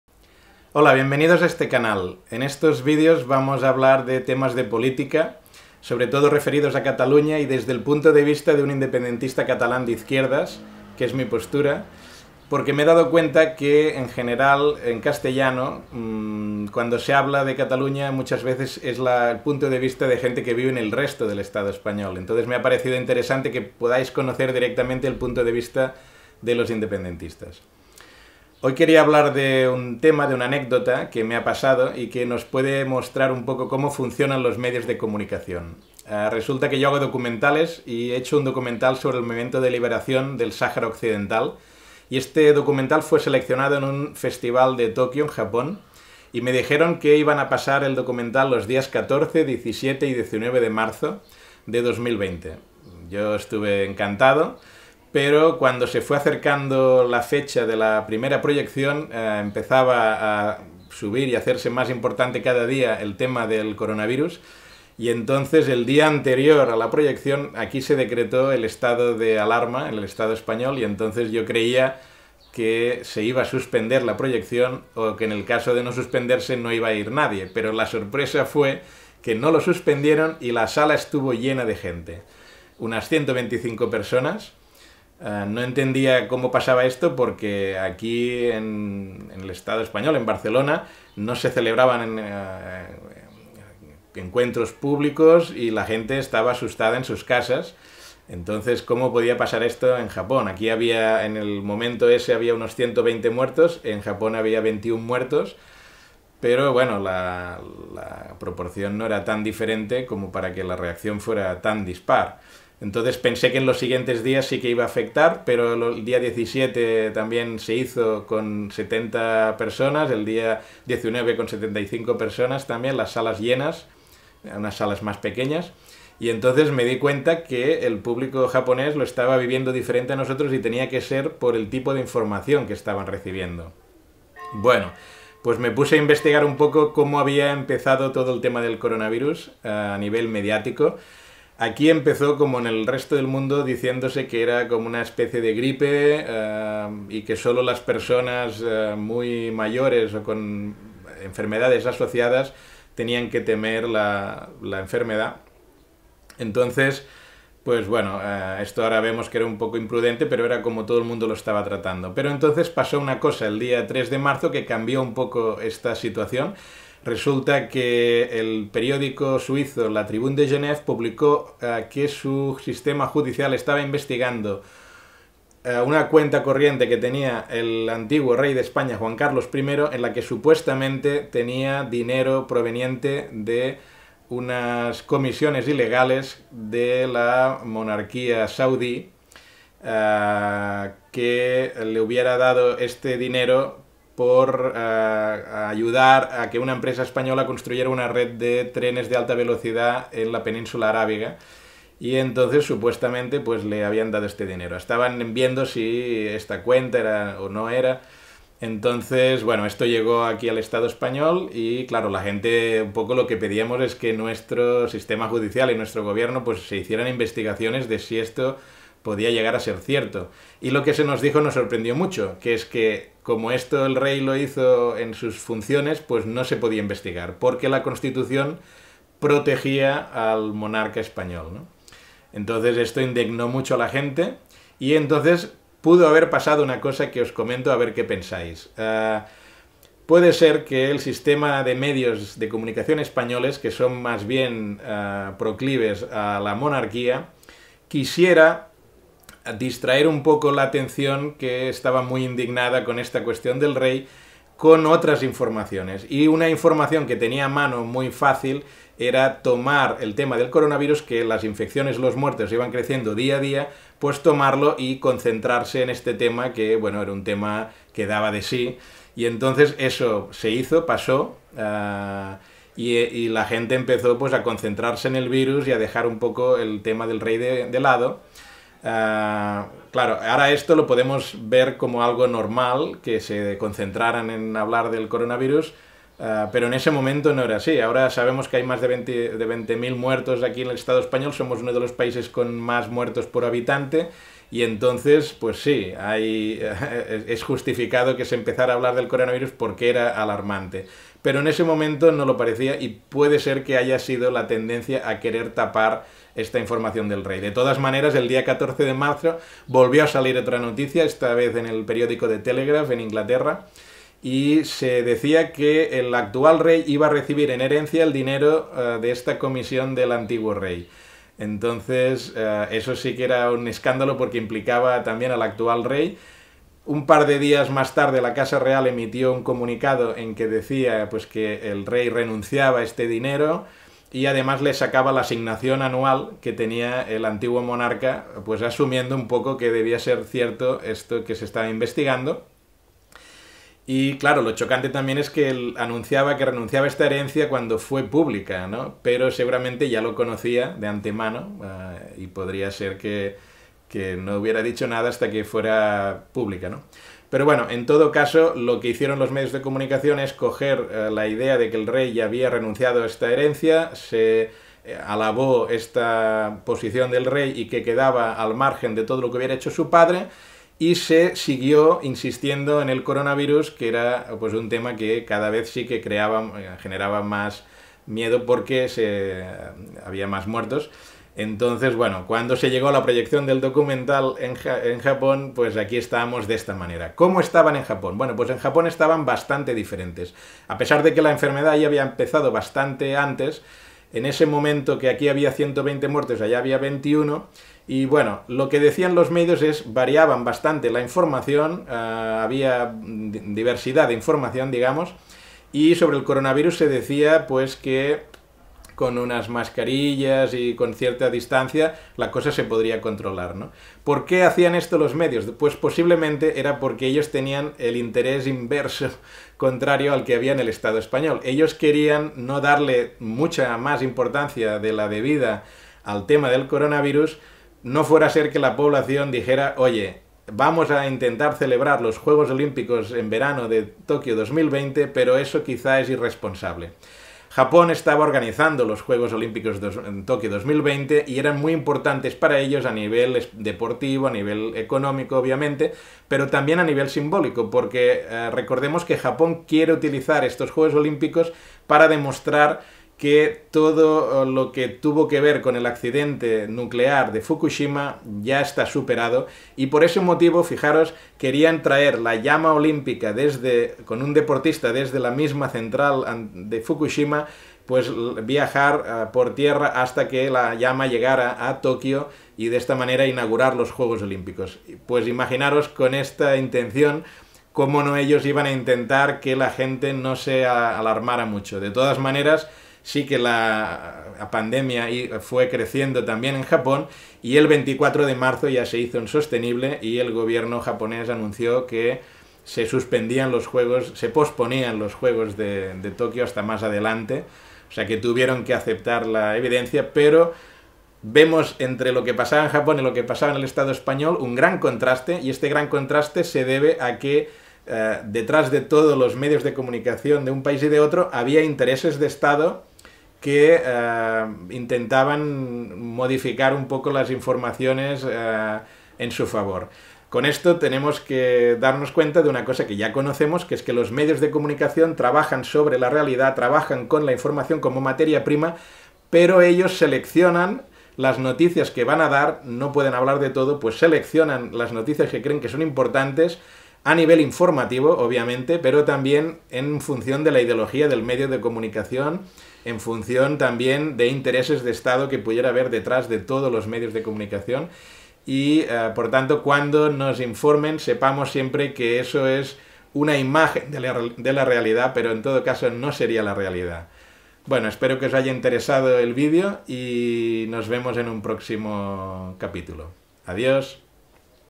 Coronavirus y mass media (en castellano)
Video 1 del canal Calidoscòpic:
Una anécdota ilustrativa sobre los medios de comunicación en tiempos de coronavirus.